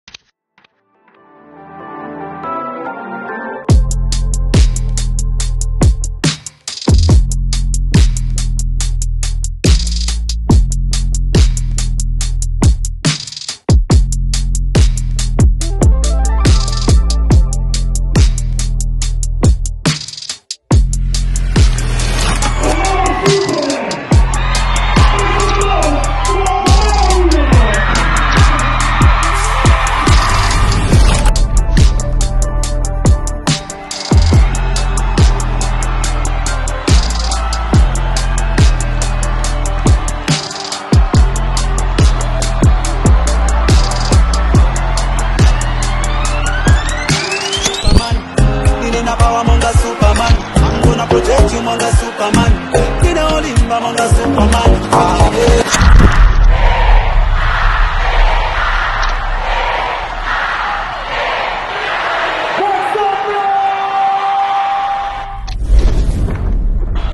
Zambia! 🇿🇲 What an experience! 🙏🏾
Stanbic Music Festival in Lusaka was nothing but vibes! The energy, the love, the culture—truly one of the greatest moments of my journey. Gratitude for welcoming me with open arms. Until next time, Africa! 🌍❤️